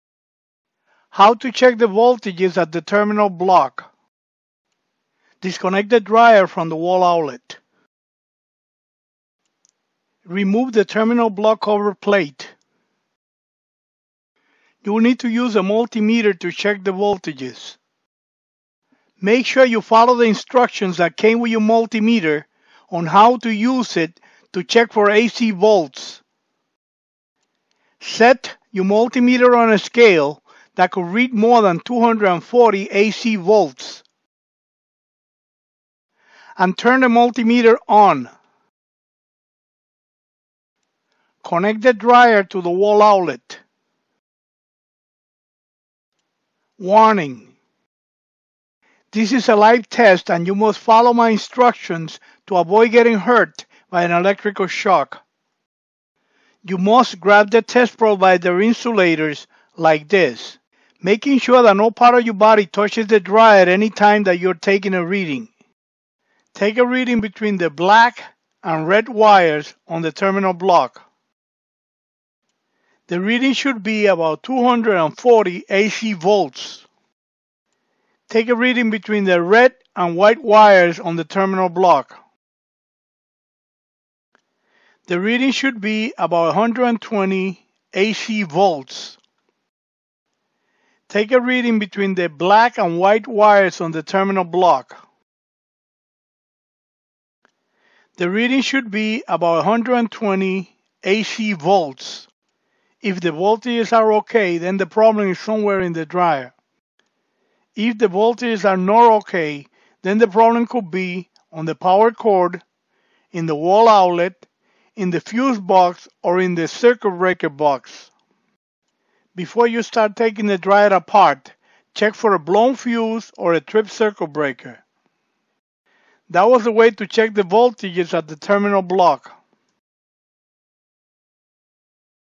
Whirlpool Dryer Not Getting Hot - Voltages at the terminal block
This video will show you how to check the voltages at the terminal block